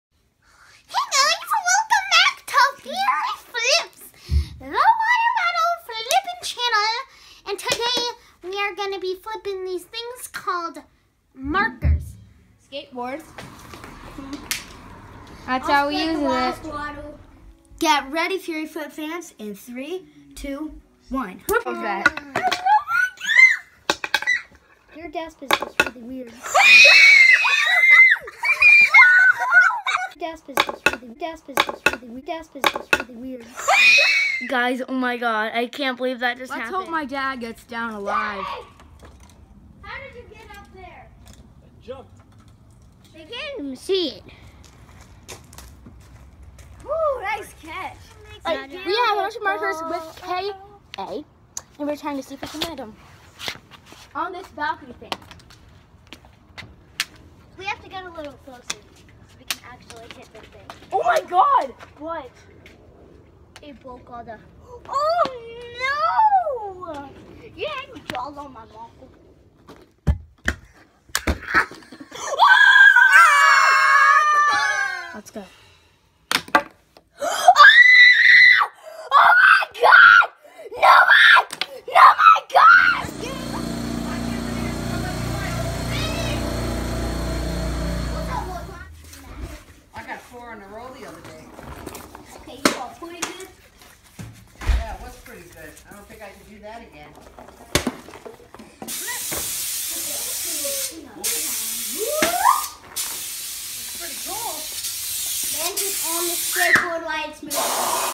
The Most INSANE Marker Flip | Cute Baby Clips
Me, Kr and Ke are going to flip a MARKER. Cute baby clips at the end.